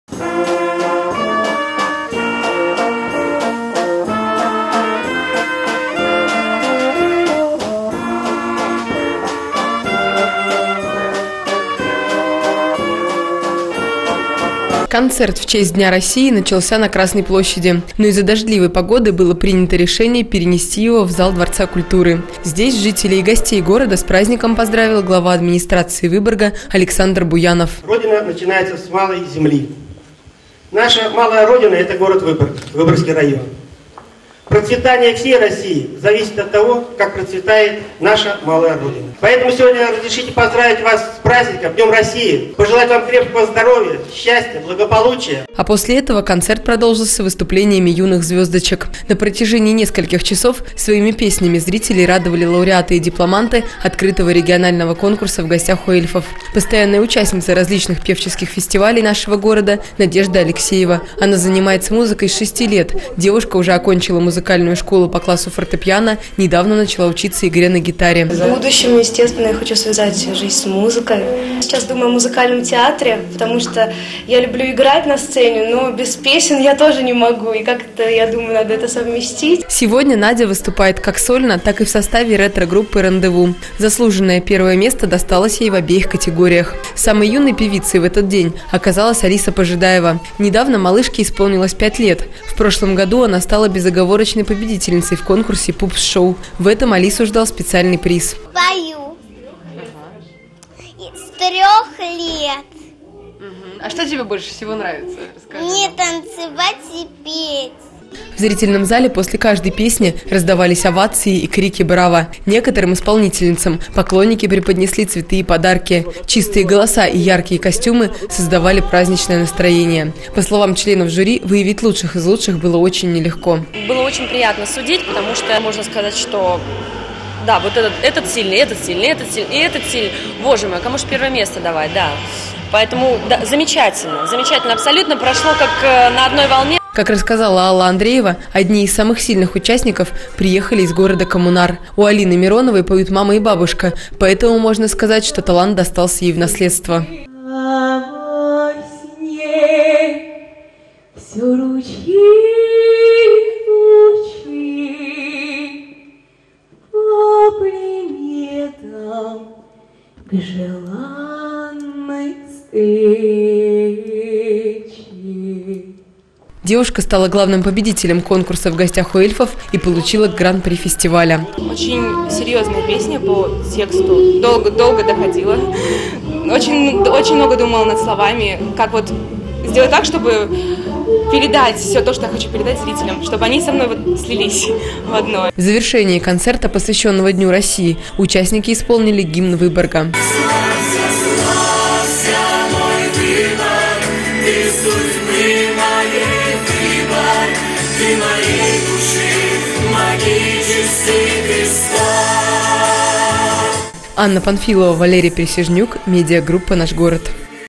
Ретроспектива-2012: концерт в честь Дня России
Девять лет назад с Днем России выборжцев поздравили лауреаты и дипломанты конкурса "В гостях в Эльфов". Сюжет выходил в эфир 13 июня 2012 года.